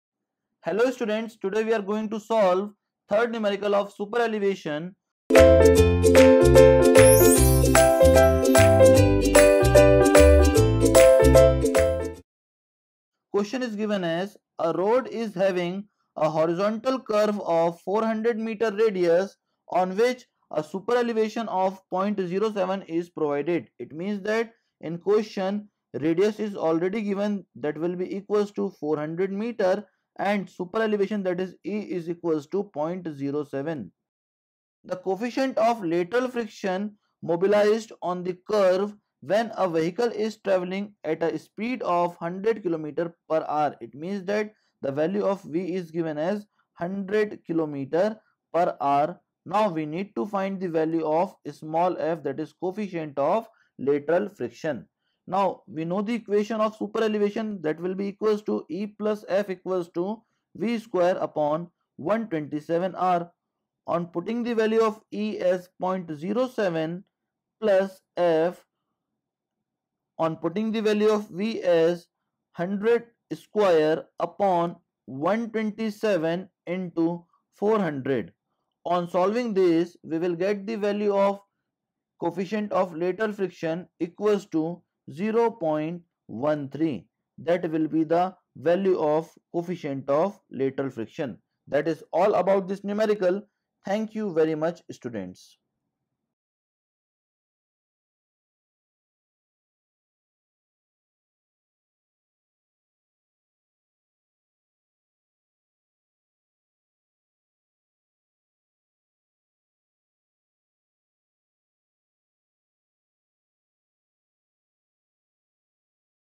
3rd Numerical of Superelevation - Highway Geometric Design - Transportation Engineering 1
Subject - Transportation Engineering - I

Video Name - 3rd Numerical of Superelevation

Chapter - Highway Geometric Design

Access the Complete Playlist of Subject Transportation Engineering - I  -

Happy Learning